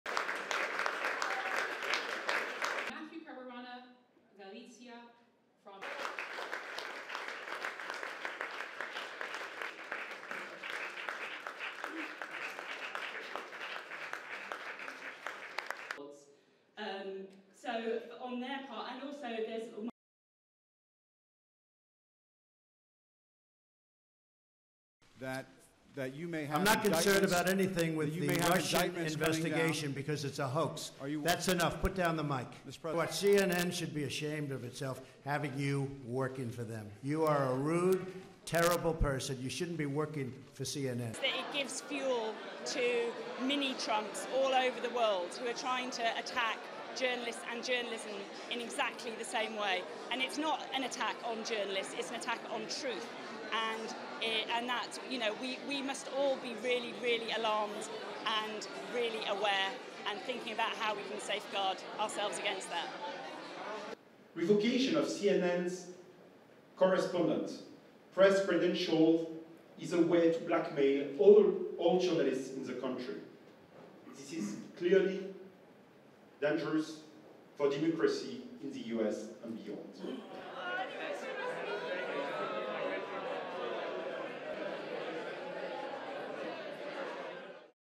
Press freedom award winners denounce Trump CNN move
More videos on Rappler Follow Rappler for the latest news in the Philippines and across the globe. The head of Reporters Without Borders (RSF) and RSF award winner Carole Cadwalladr denounce Donald Trump’s move to remove media credentials from CNN White House journalist Jim Acosta. The comments were made at RSF’s annual press freedom awards, held in London for the first time, where Cadwalladr won ‘L’Esprit de RSF’ award for her work on Brexit. Related story Follow Rappler for the latest news in the Philippines and around the world.